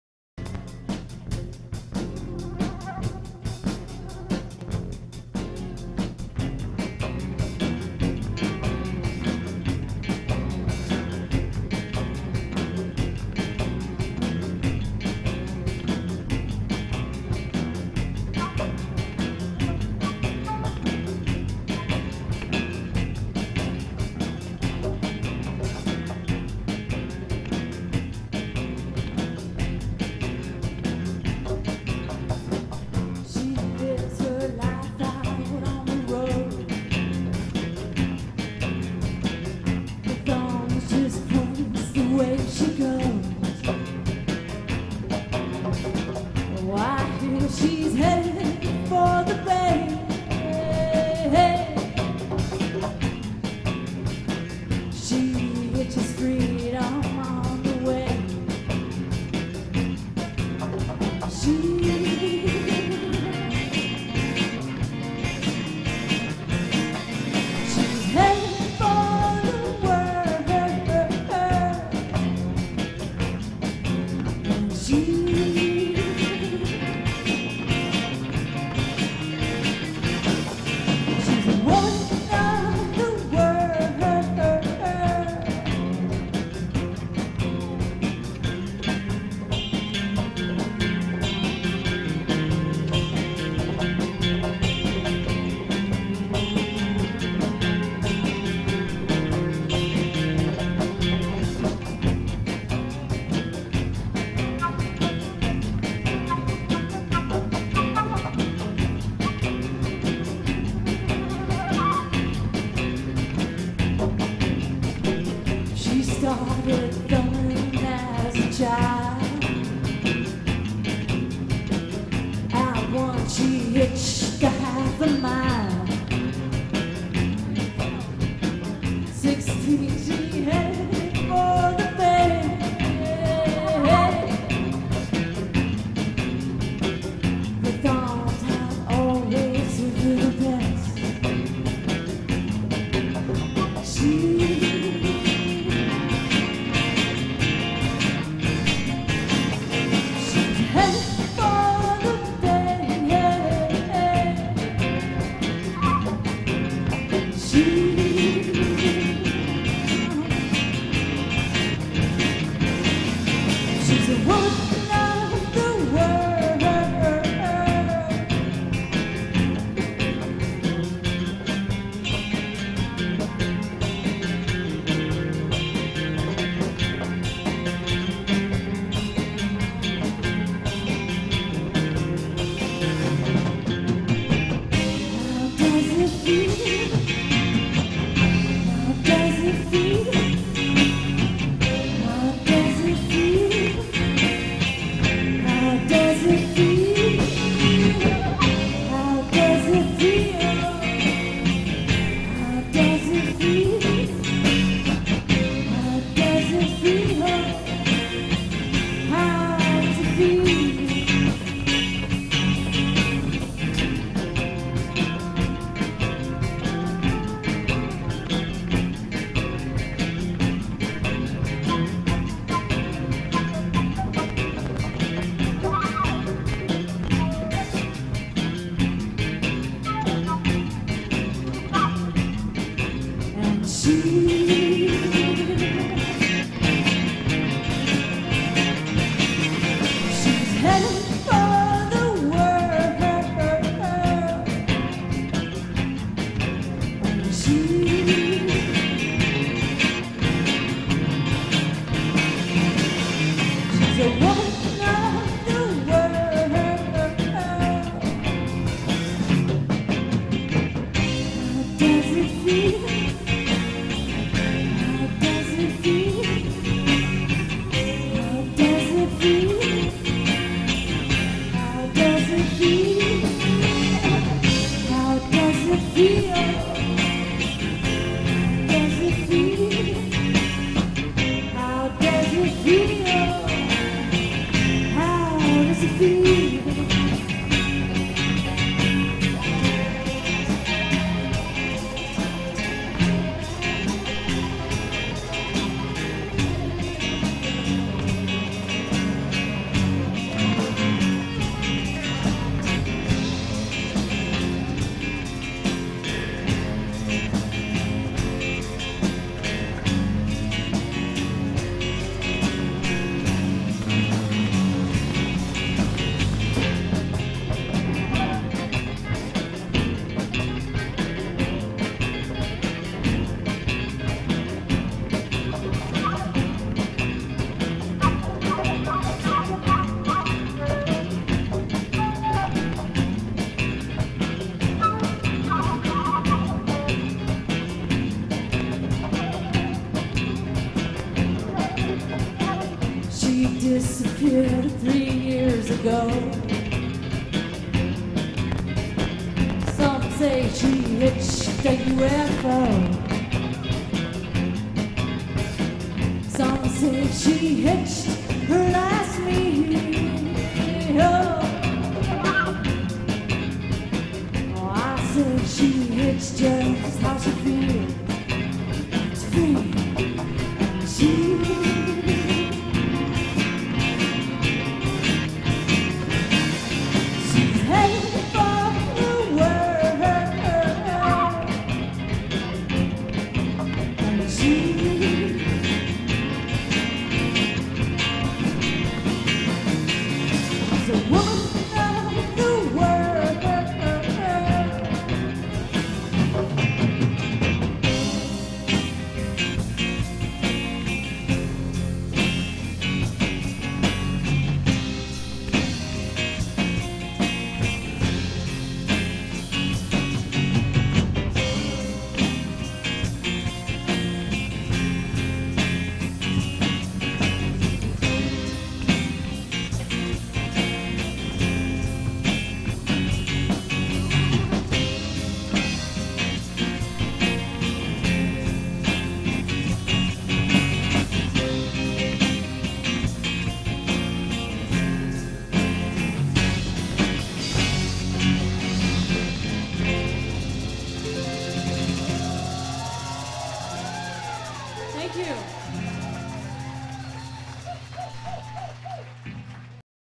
Trippy video of the Clay People @ The River Farm in Van Zandt, WA (1995)
I don't remember how this happened, but I wanted to save the performance, so I'm sure the slow roll happened when recording to another VCR along with the audio from my 4-track cassette recording that was done simultaneously.
The audio is really good, although the signal is a bit low. Some smooth bass playing by the late Bill Sterling, who also produced the event, and haunting and vocals by Cheri Nunamaker. I will have to fill in info on the other musicians later.
*Sound engineer - Mike Thomas
*Live audio mix and video by Jeff Markowicz (Rhythmn Productions)
*Event producer - Bill Sterling (RIP)